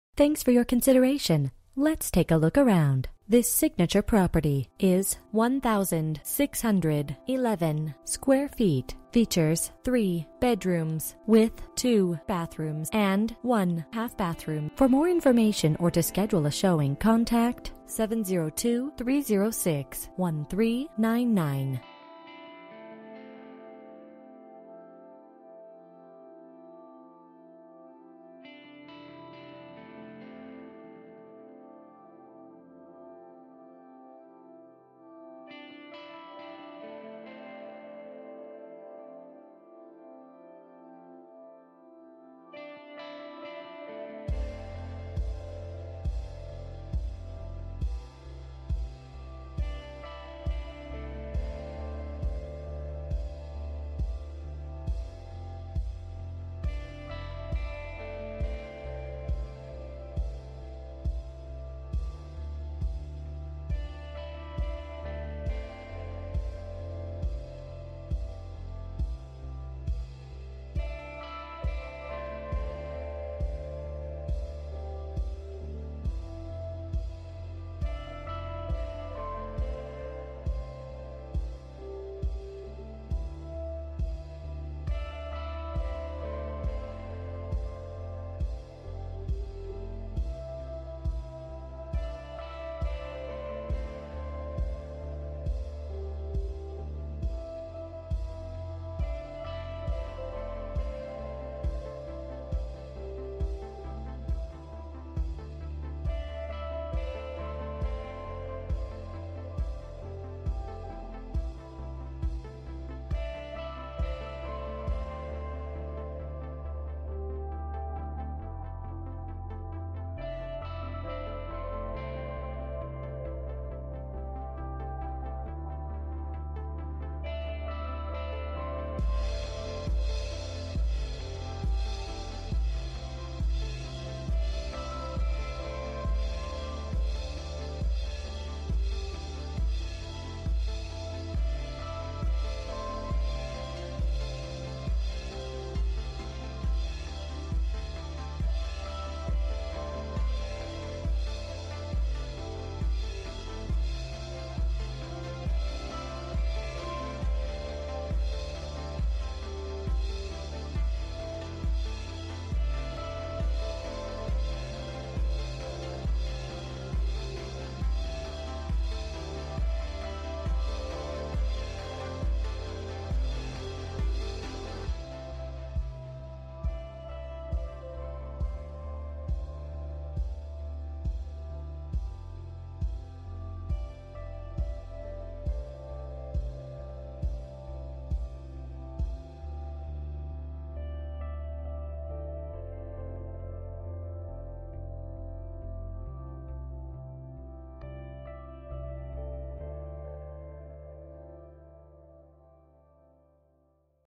New Residential listing for sale found at 9124 Bushy Tail Ave unit 103, Las Vegas, NV 89149
Bedrooms: 3
Bathrooms: 2 full and 1 half
Square Feet: 1611
Price: $346,000